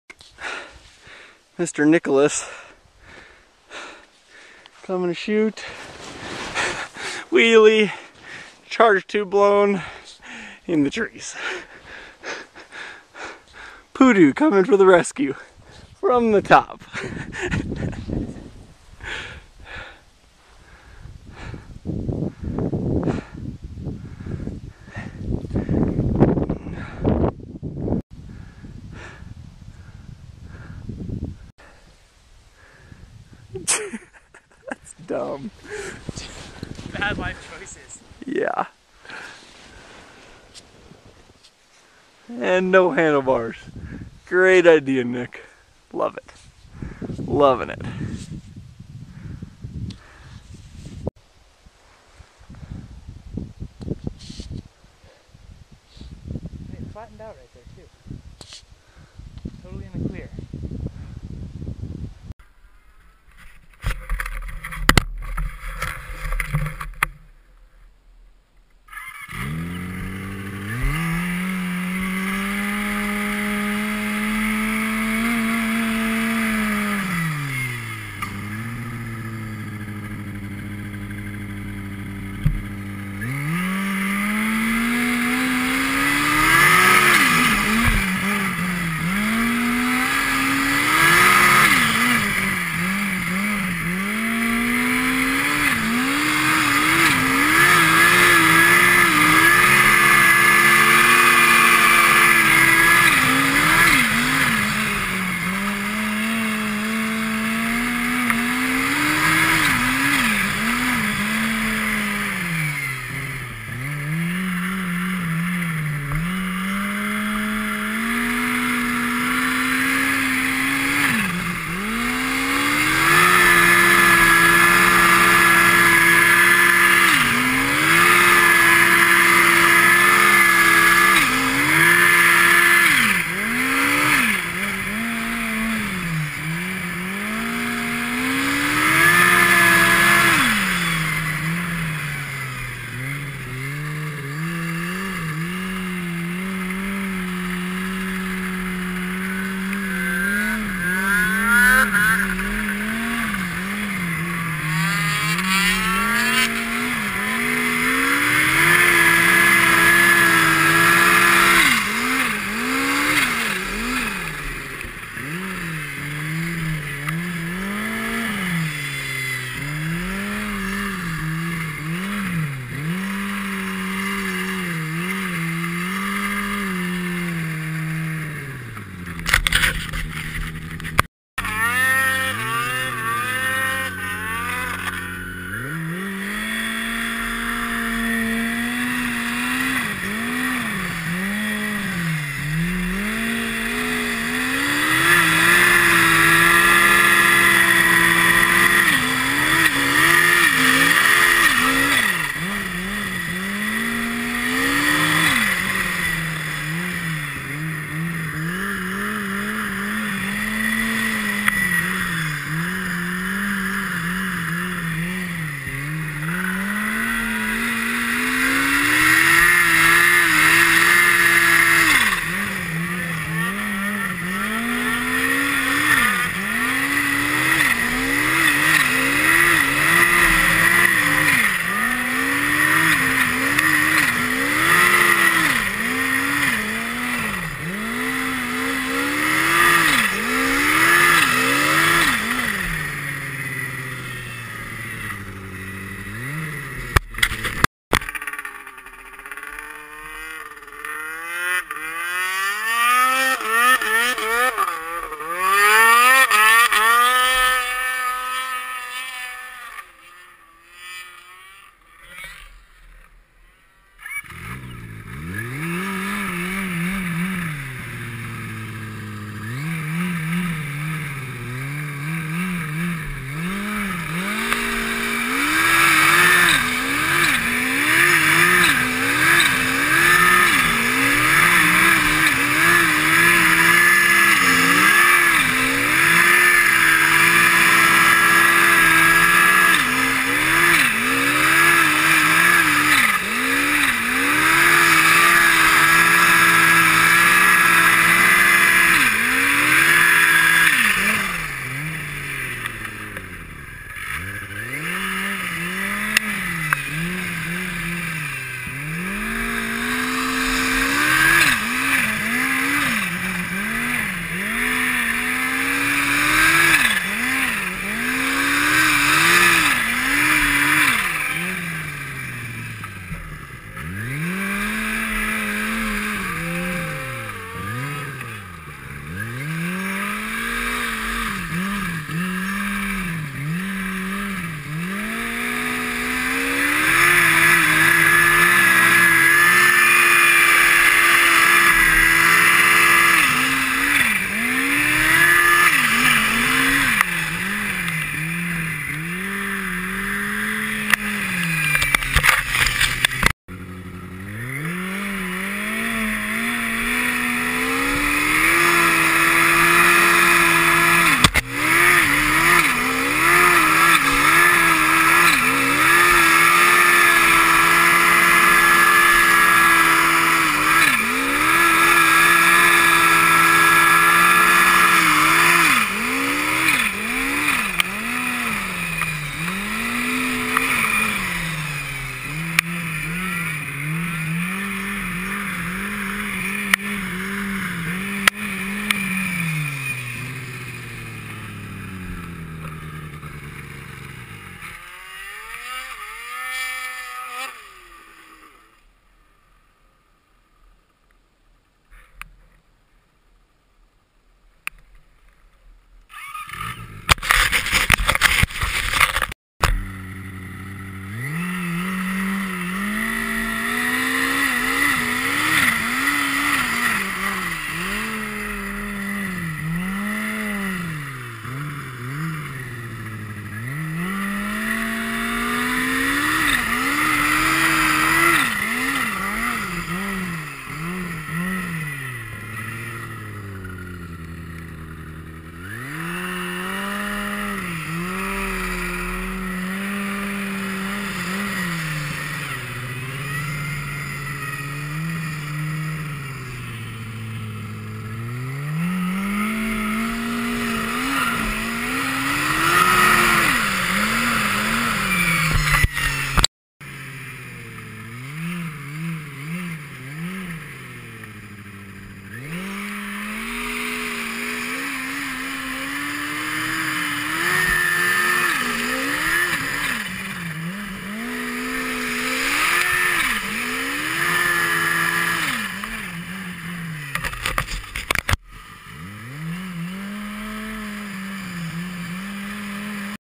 Snowmobiling brackett creek on the turbo's
josh on the poodoo
turbo 800 poo motor in a custom tube chassis.

me on the yamadoo
propane fueled turbo rx1 motor in a doo chassis

little incident at the begining, the charge tube blew off right when i tossed it into the sidehill, so no power = nasty roll into the trees and josh accidentally hit the bump in the bowl while staring at his gauges... busted up elbow, but all was good.